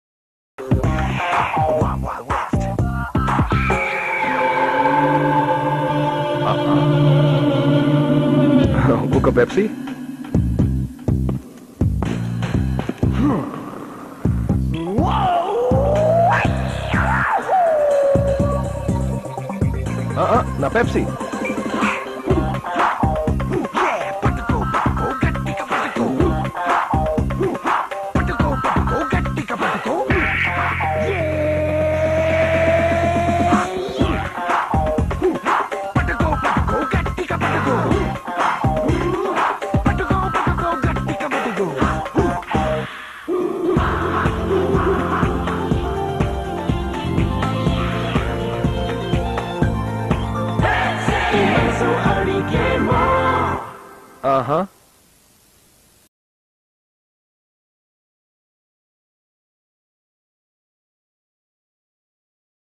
20 ఏళ్లలో పవన్ కళ్యాణ్ చేసిన ఏకైక AD | Pawan Kalyan One & Only Pepsi AD In Entire 20 Years | FF Buzz
20 ఏళ్లలో పవన్ కళ్యాణ్ చేసిన ఏకైక AD | Pawan Kalyan One & Only Pepsi AD In Entire 20 Years | FF Buzz